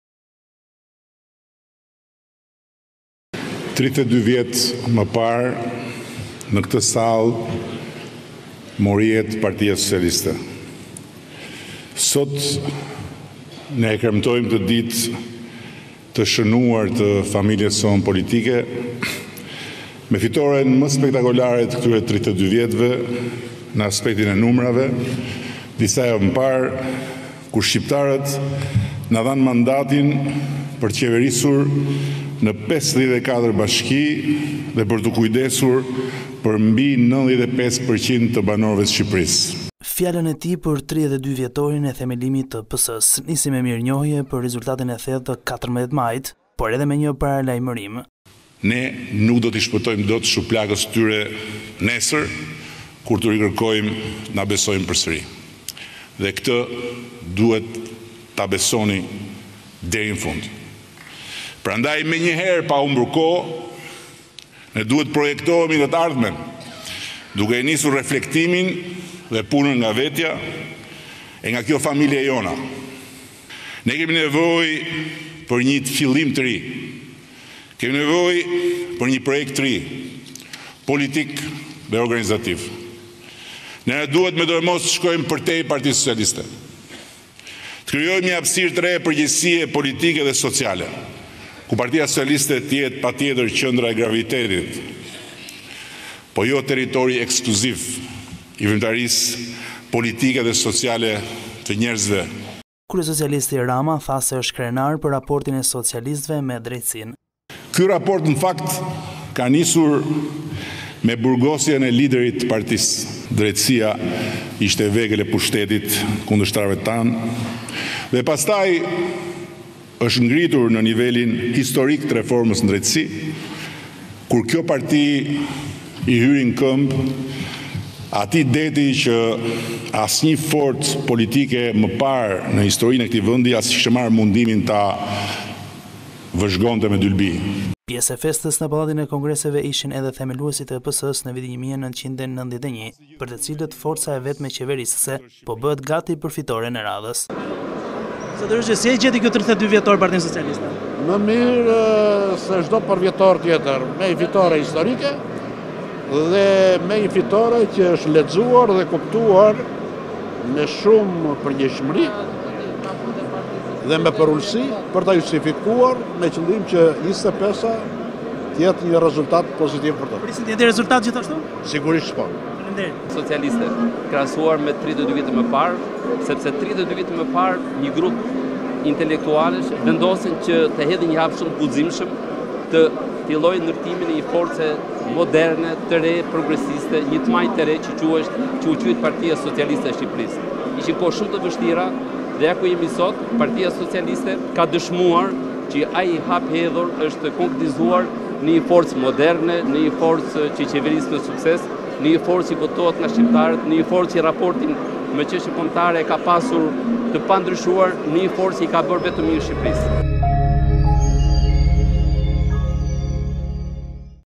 32 vjet Parti Socialiste/ Rama: Karikojmë bateritë për 2025! Mbyllni shtigjet e vjetra
reportTV lajmeshqip lajme Mes kontribuesve të rinj e të vjetër, Partia Socialiste festoi sot 32-vjetorin e themelimit të saj në Pallatin e Kongreseve. Ishte kryesocialisti Edi Rama, i vetmi i cili mbajti një fjalim të gjatë, të cilin e nisi me falënderime për fitoren historike që korri në zgjedhjet e 14 majit e më pas vijoi projektimin për sfidat politike të PS-së në të ardhmen